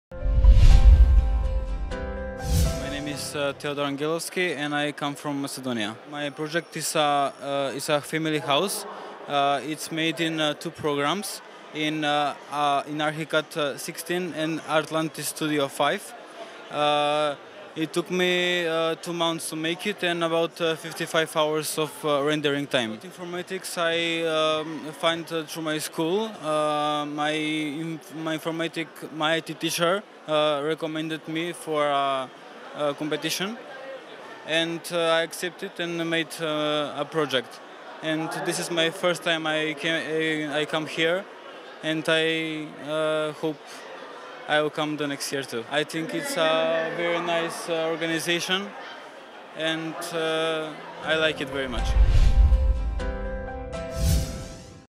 Infomatrix 2014 World Finals // Modern House by Teodor Angelovski
Project Name : Modern House
Category : Computer Art
Country : Macedonia
Made by : Teodor Angelovski